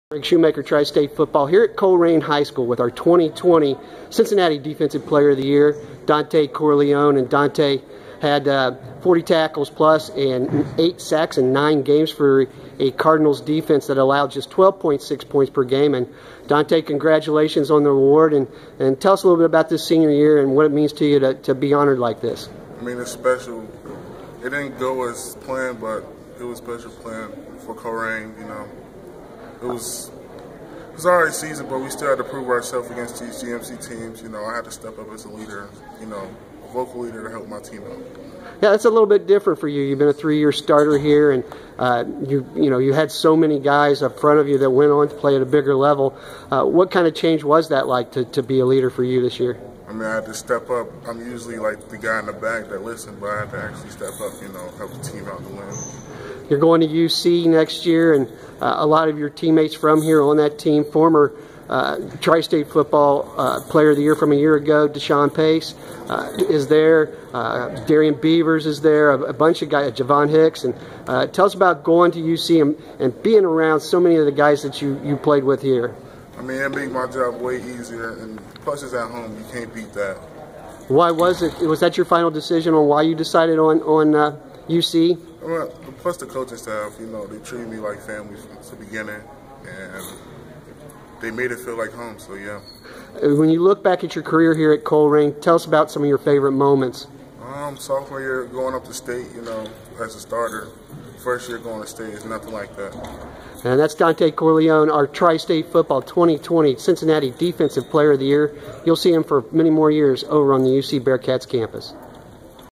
2020 TSF Awards: Dontay Corleone, Colerain
Colerain DL Dontay Corleone talks with Greg Shoemaker of TriState Football about being selected the 2020 TSF Cincinnati Defensive Player of the Year. Corleone was in on 42 tackles and had eight sacks as a senior for a Cardinals defense that allowed just 12.6 points per contest.

Corleone, a University of Cincinnati commit was also named OPSWA Division I First Team All-Ohio, First Team All-TriState Football (for a second straight year) and the GMC Defensive Player of the Year.

TriStateFootball.com is the only media outlet that covers Southwest Ohio, Northern Kentucky, and Eastern Indiana high school football year round.